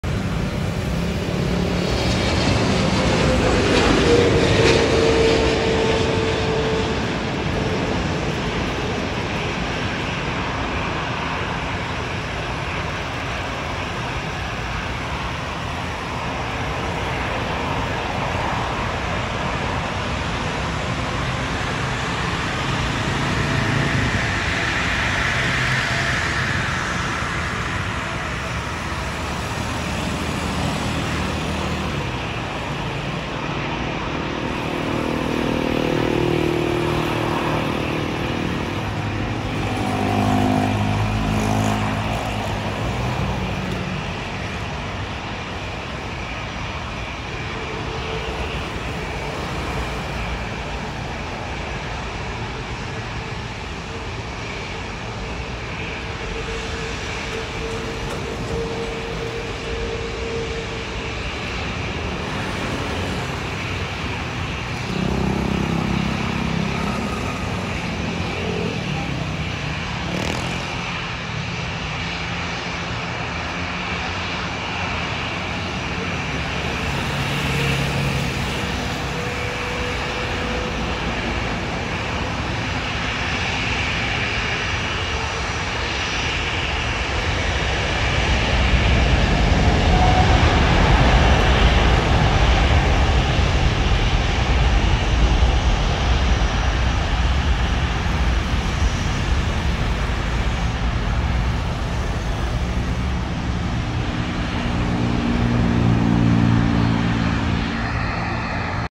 Pal 777-300ER landing and Gulf Air 787-9 departure from Manila runway06
Recorded earlier 11am